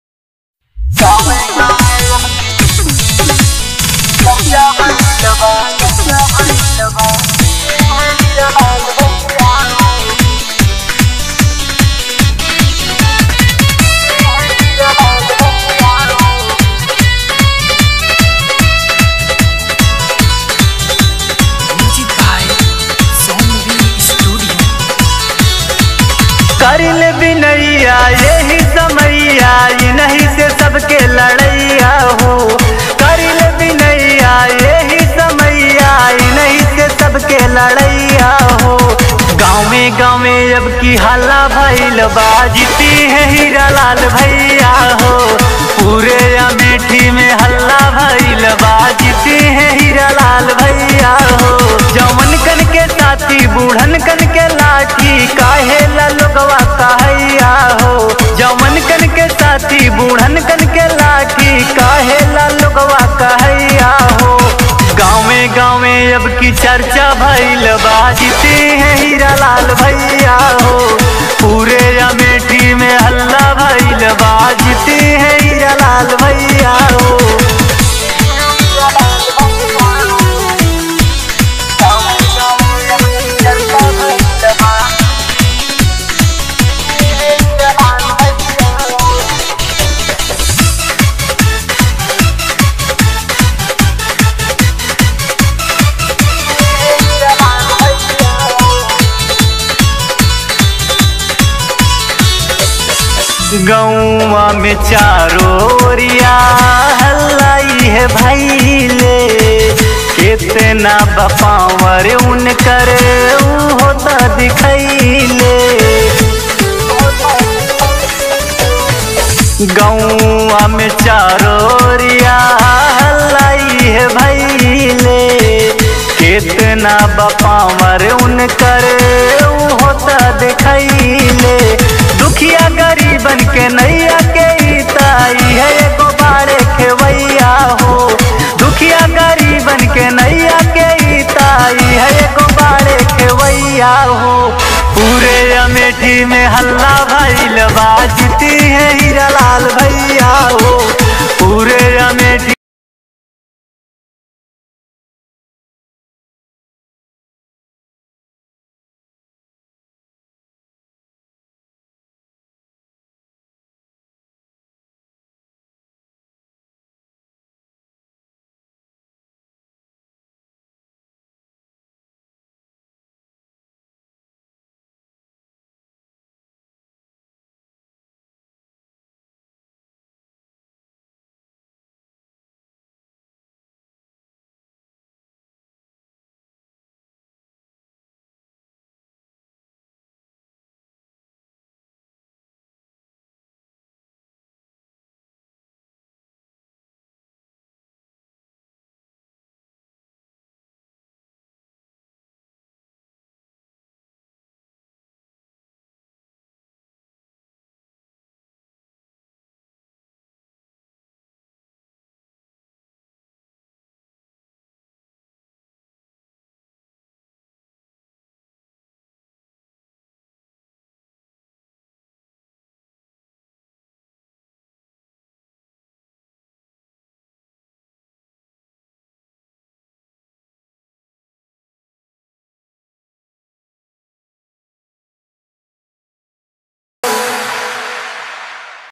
विधानसभा 186अमेठी समाजवादी भावी विधायक हीरालाल यादव Manish Mastana Kundaचुनाव गीत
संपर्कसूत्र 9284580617

सिंगर मनीष मस्ताना

रिकॉर्डिंग करन सनेहिया